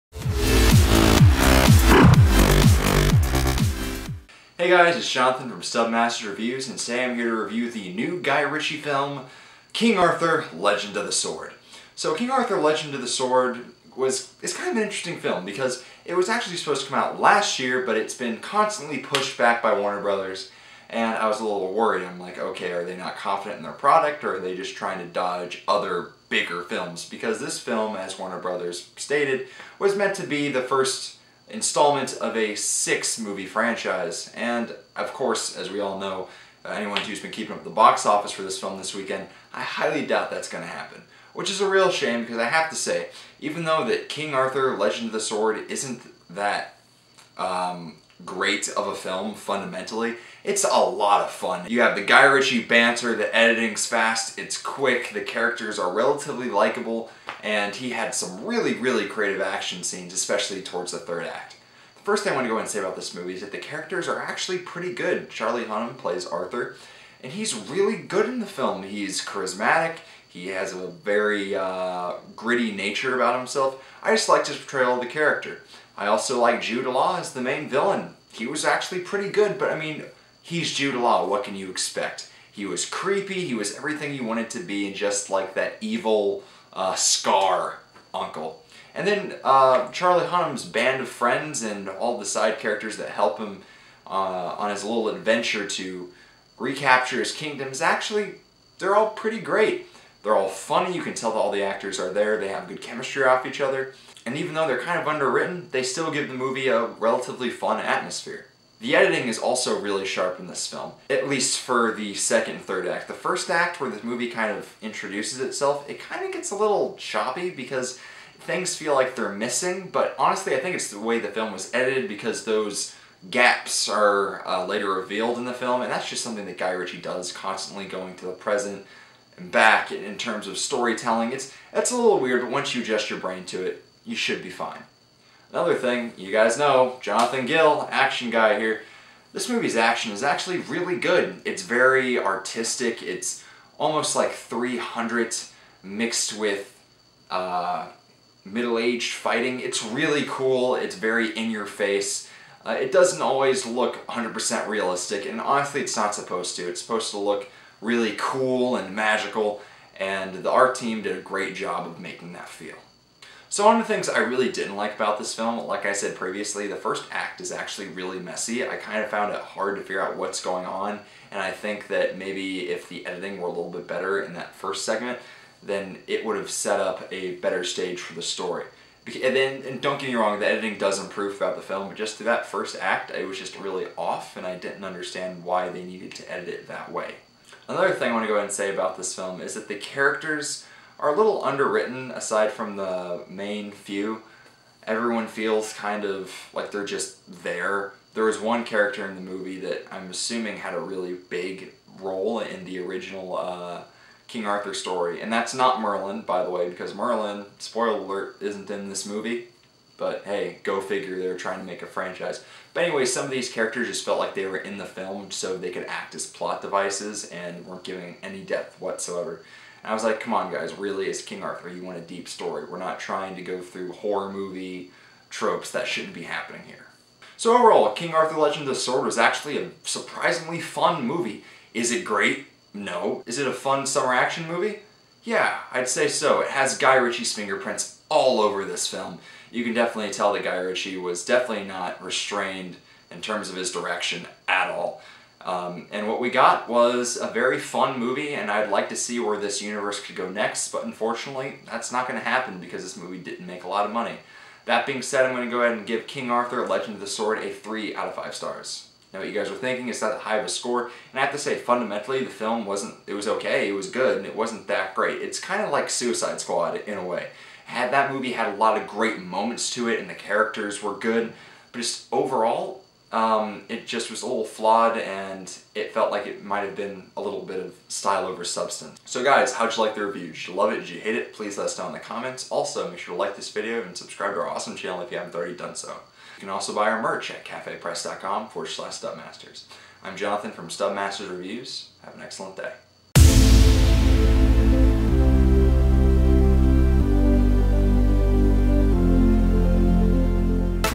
King Arthur: Legend Of The Sword Review
Guy Ritchie makes a flawed yet enjoyable spin on a classic tale.